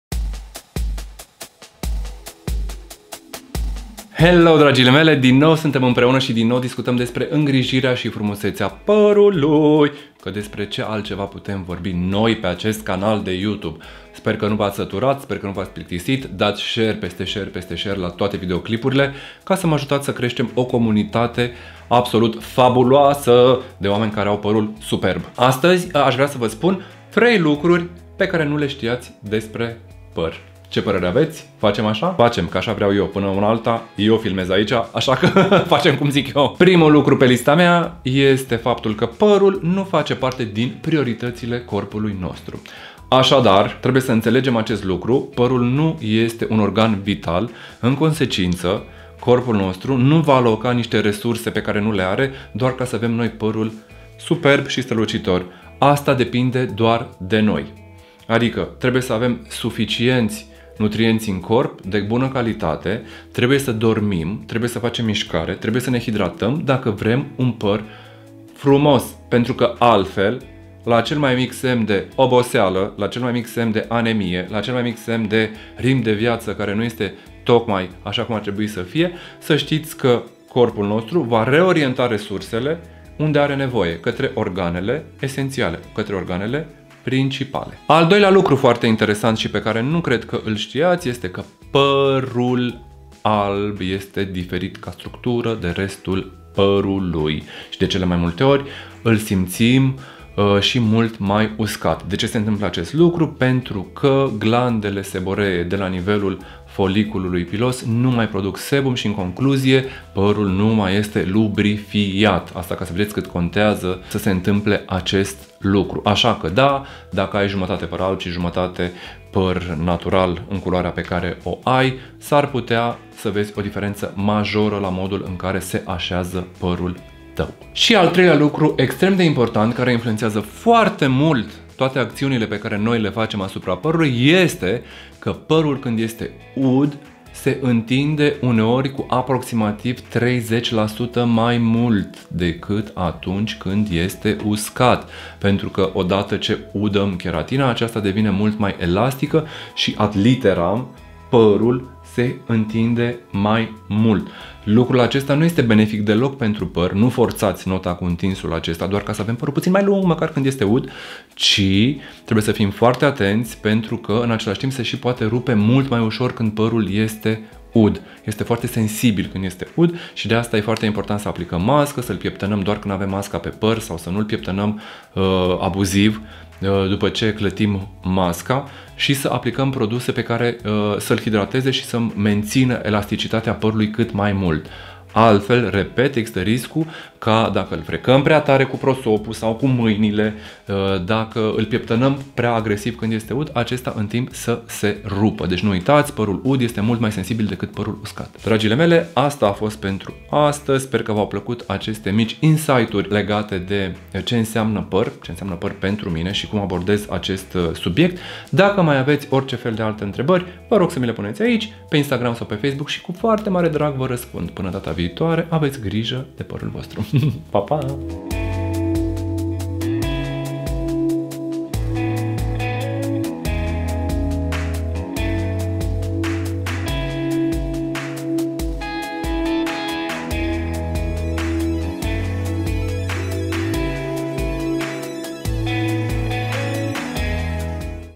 3 lucruri pe care nu le stiati despre par
Astazi vorbim despre 3 lucruri pe care nu le stiati despre par. Sper ca vi s-au parut interesante si ca v-au fost de ajutor. Nu ezitati sa imi adresati orice alta intrebare atat aici in sectiunea de comentarii, cat si pe Instagramul meu.